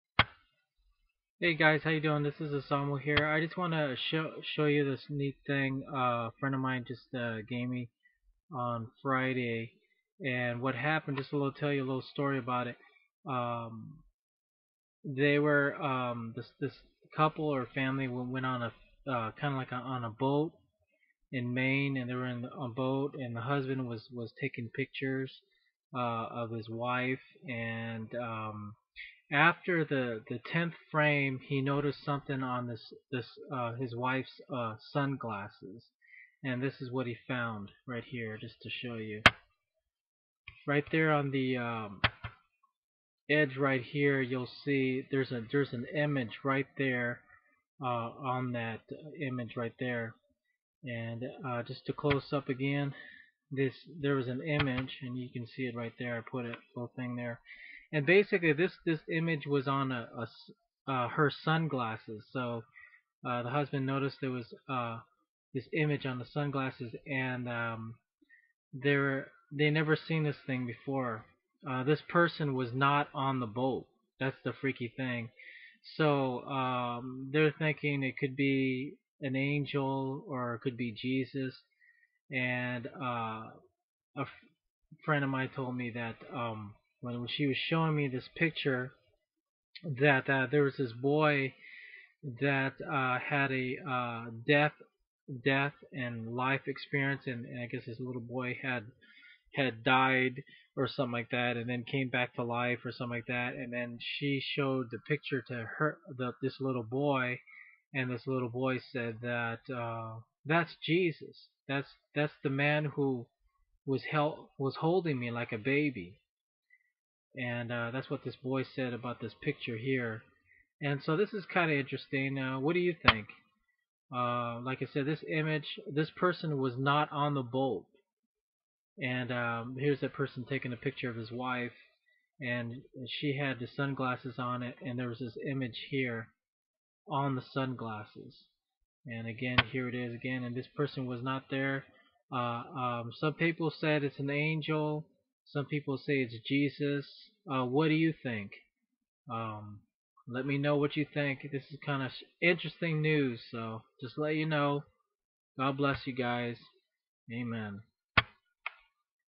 Must See Jesus or Angel Caught on Film on Sunglass
This Image of a Man in Gray appeared on a woman's sunglasses during a photo shoot. Story goes a family was in a boat. The husband was talking pics of his wife and after the 10th frame there was a imag